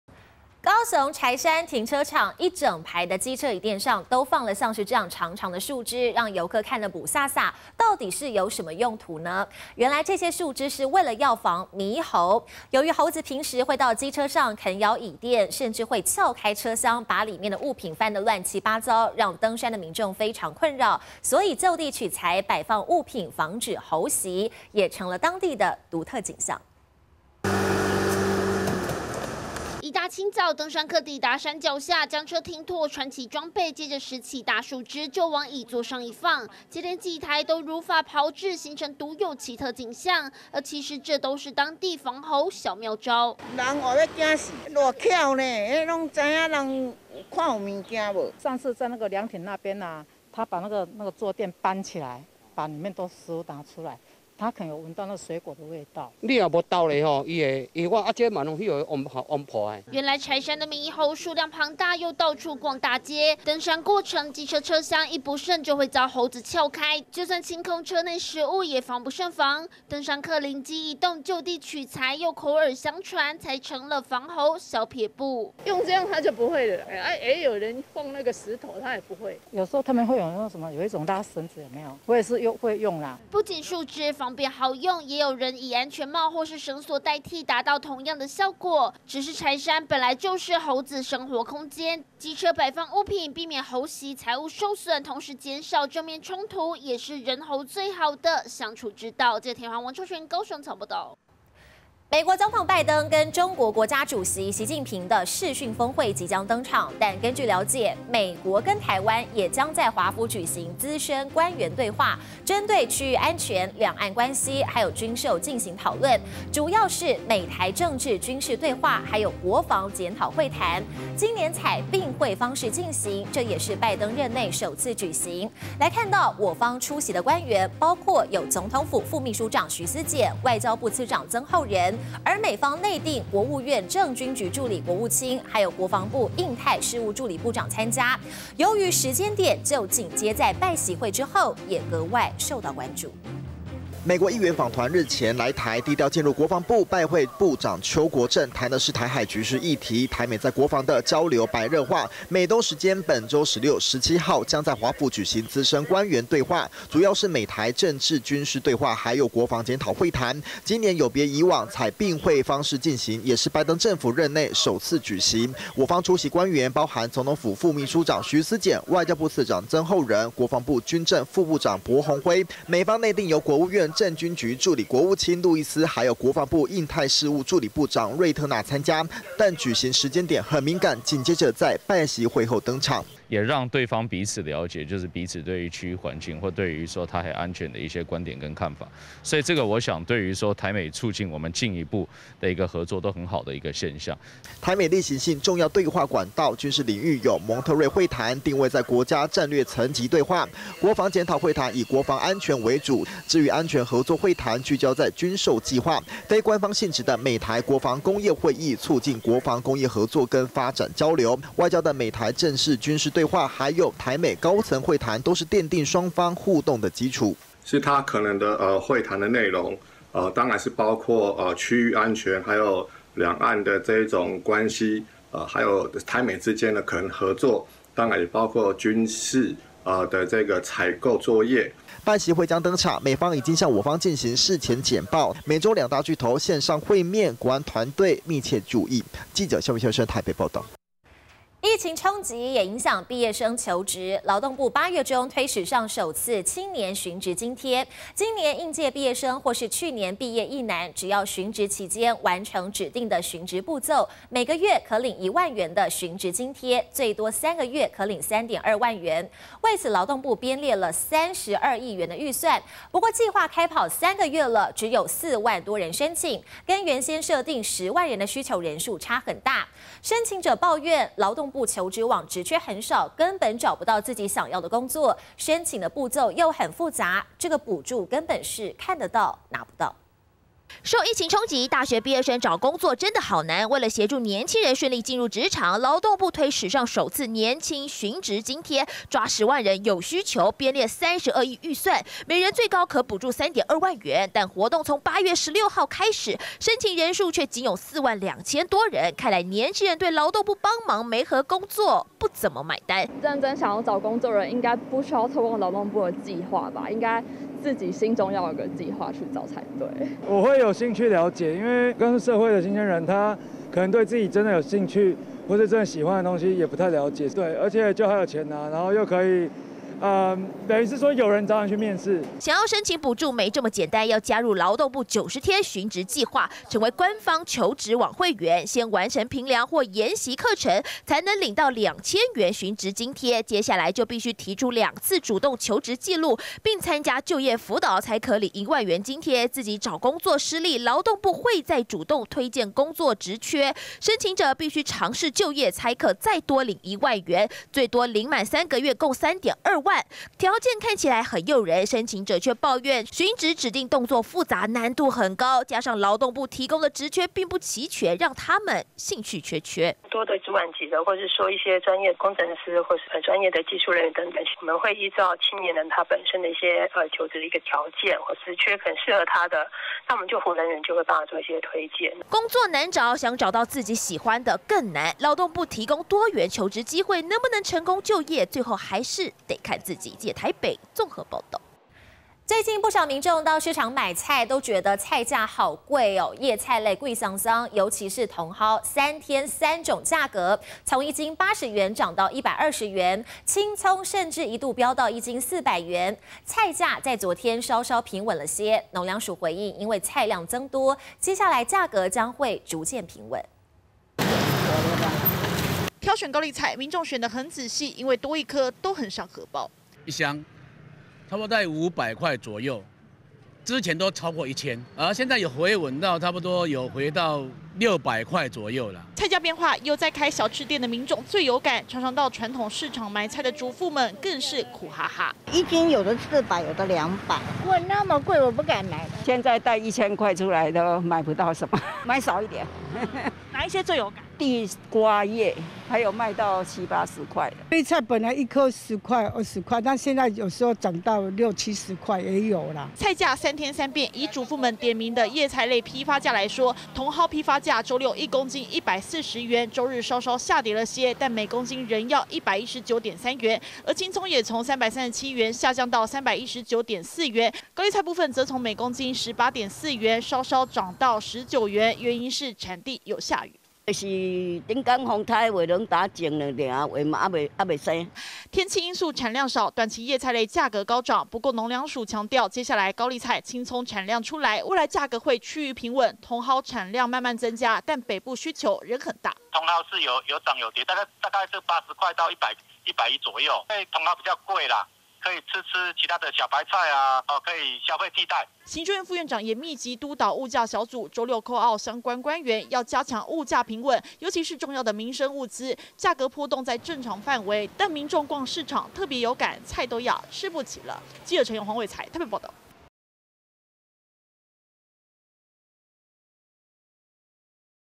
機車插超長樹枝 柴山「奇景」！當地人曝原因【重點新聞】-2021115
(00:00)「別來咬」 機車椅墊放棍嚇猴成當地奇景
(01:41)拜習視訊會後 台美舉行資深官員對話
(04:01)新鮮人喊苦！ 史上首次青年尋職津貼「吃不到」
(06:22)菜好貴買不下！ 茼蒿3天1斤80漲到120元